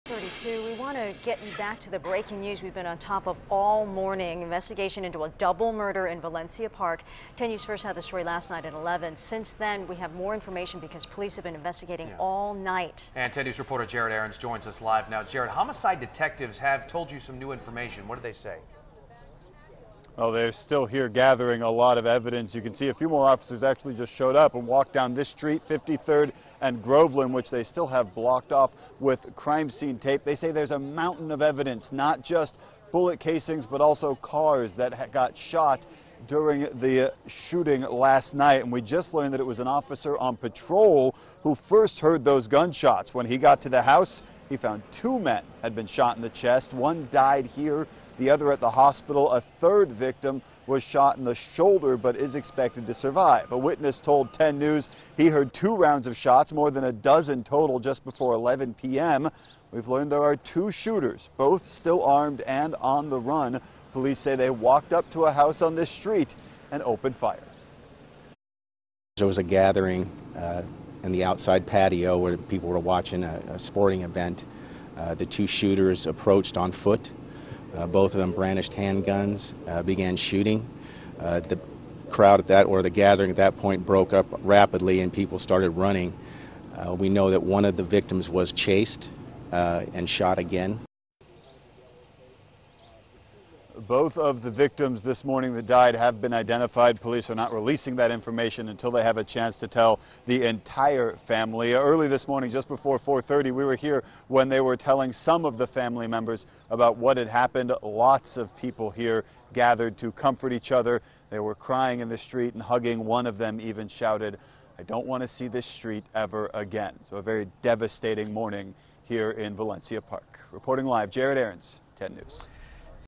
Two killed, 1 injured in Valencia Park shooting
Two people are dead and a third is injured in a shooting late Monday night in the Valencia Park neighborhood of San Diego.